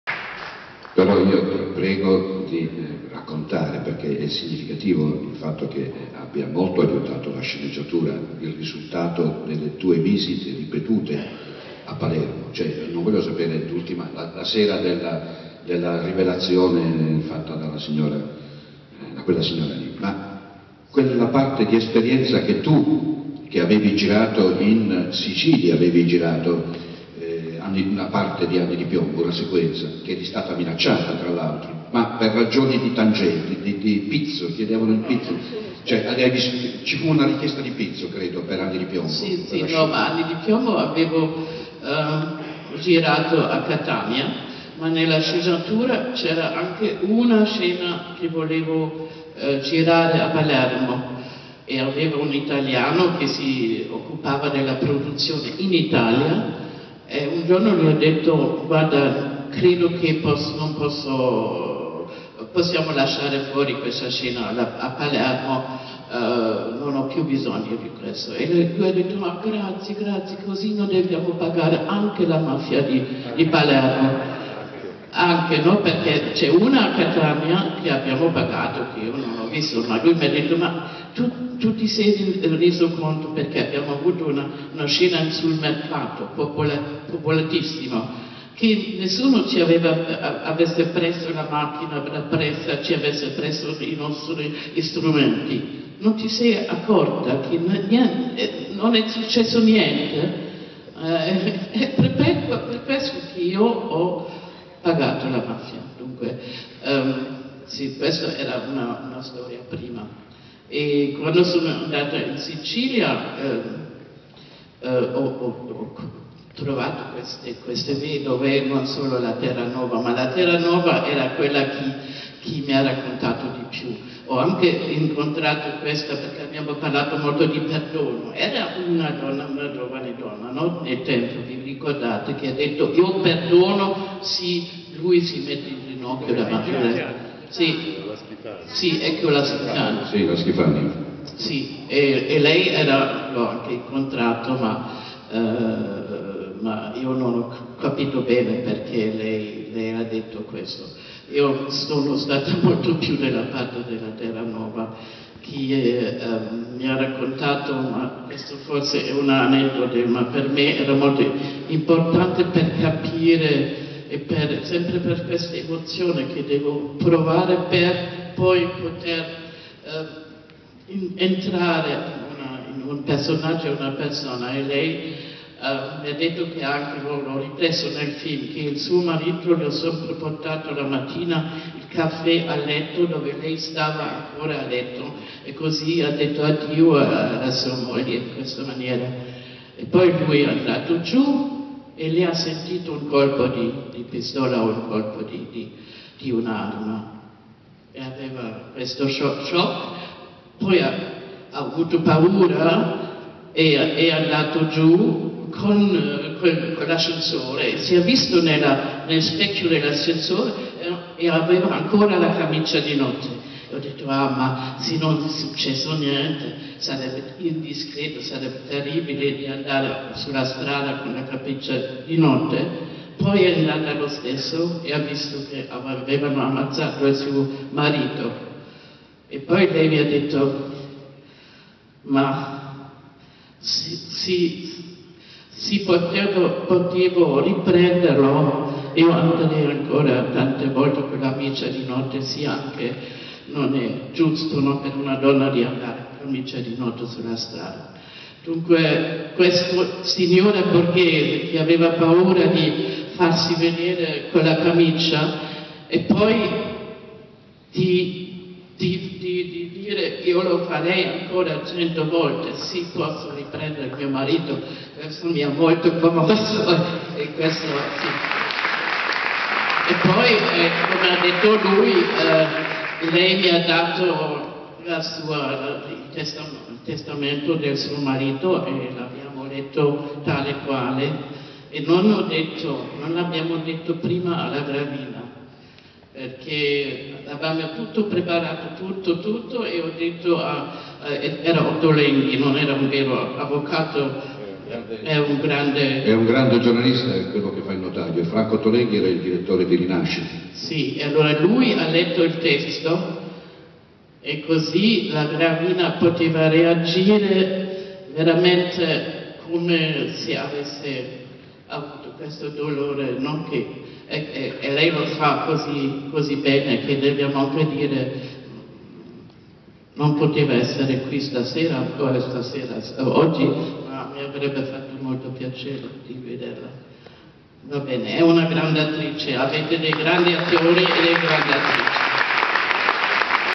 Margarethe Von Trotta al BIF&ST 2018: i retroscena di Anni di Piombo e de Il lungo silenzio
- In questo video Margarethe Von Trotta, dopo la proiezione del suo film Il lungo silenzio al BIF&ST 2018, racconta la situazione della Sicilia durante le riprese di Anni di piombo e la necessitÃ  di pagare il pizzo per stare tranquilli. Poi racconta lâ€™incontro con le vedove per Il lungo silenzio, in particolar modo con la moglie del magistrato assassinato dalla mafia Cesare Terranova con cui ha parlato di piÃ¹. Le ha raccontato, con molto dolore, lâ€™aneddoto del giorno dellâ€™assassinio del marito che lâ€™ha salutata portandole il caffÃ¨ a letto. Appena lui Ã¨ sceso lei ha sentito un colpo di arma da fuoco: ha avuto paura, Ã¨ scesa con lâ€™ascensore e ha ricordato il momento in cui si Ã¨ vista nello specchio con la camicia da notte. Il ricordo successivo Ã¨ quello del marito morto ammazzato. Il ruolo della vedova Terranova Ã¨ stato interpretato da Carla Gravina che la regista ricorda con molto affetto.